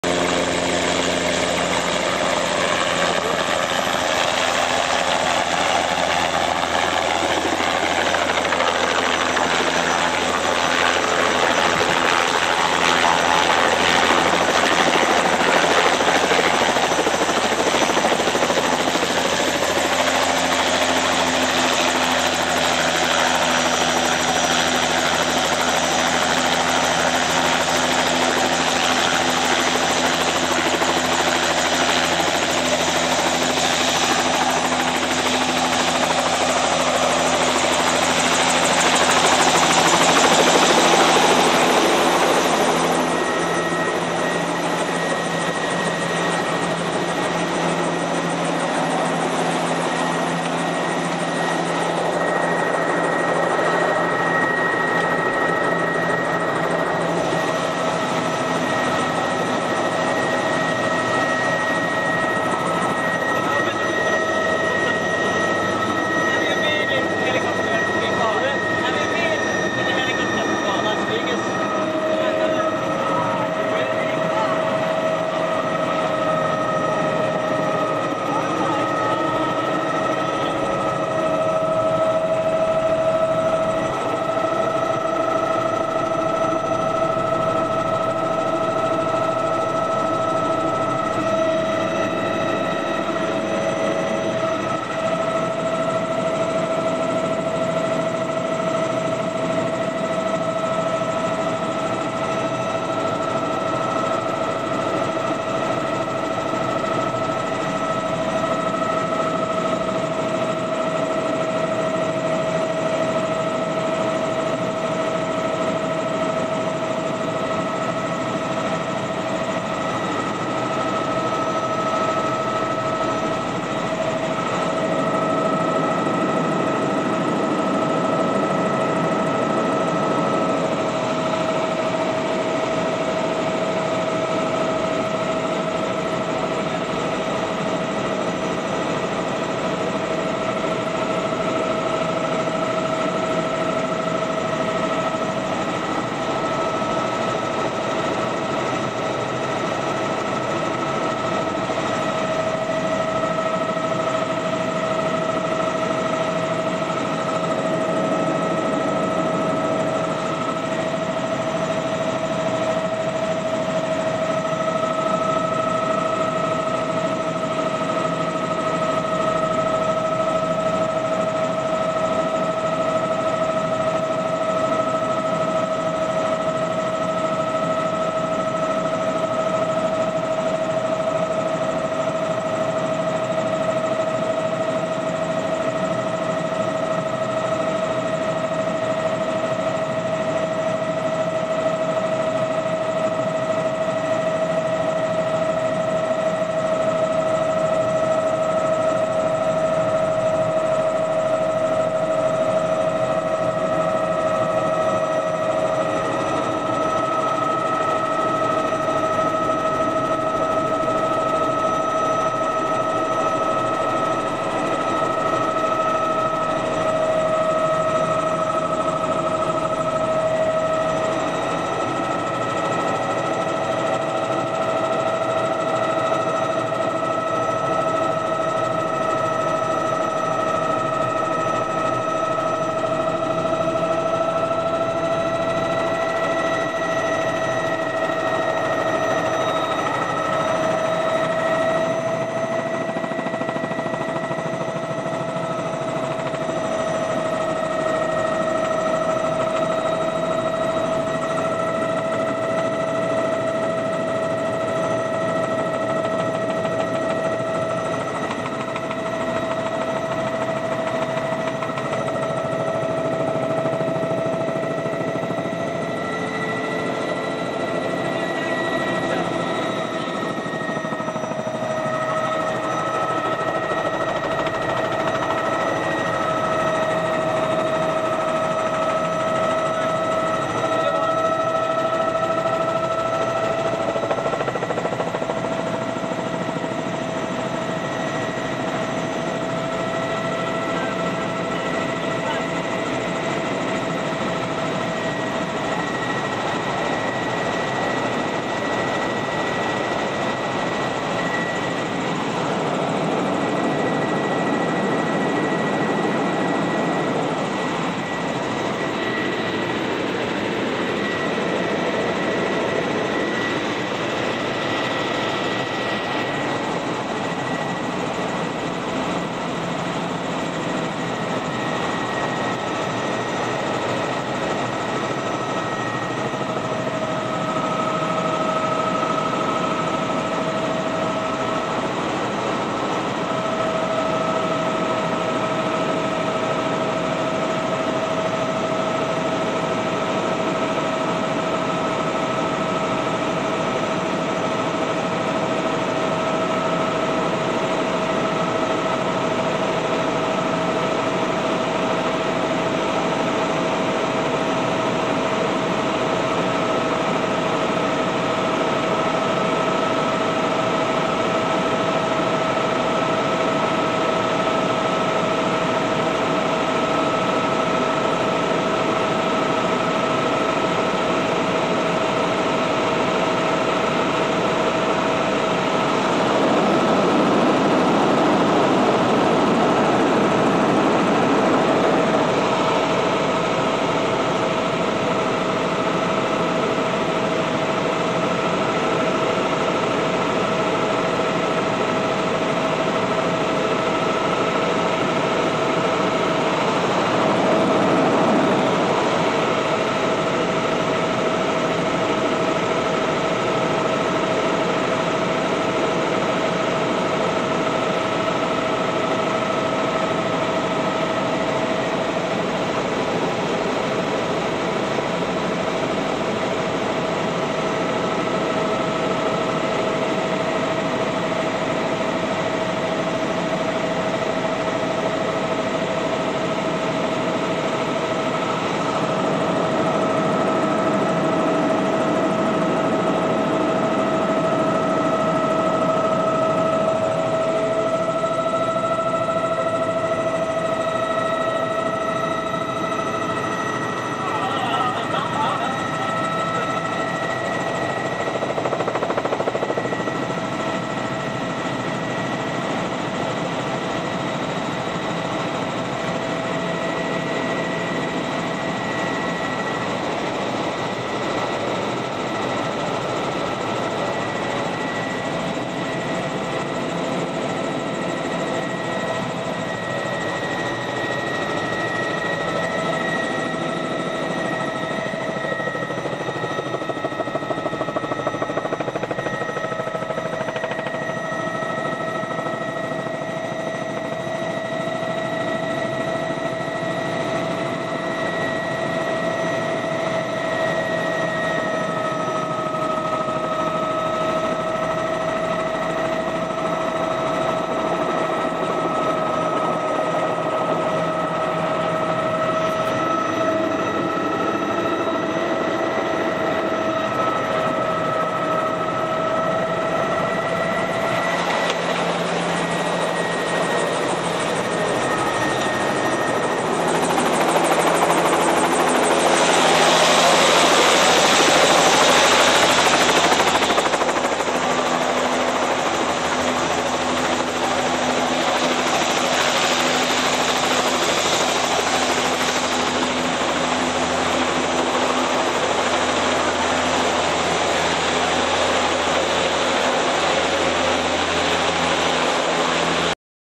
Helikopterilento Rio de Janeirossa 2006
Helicopter flight in Rio de Janeiro April 2006
11-minute guided helicopter tour over Rio de Janeiro, from Sugar Loaf Mountain to Christ the Redeemer (about 4 min). Spot also Copacabana Beach and Ipanema Beach.

I gave the front row seat to Australian man Tim, when he promised to videotape our flight.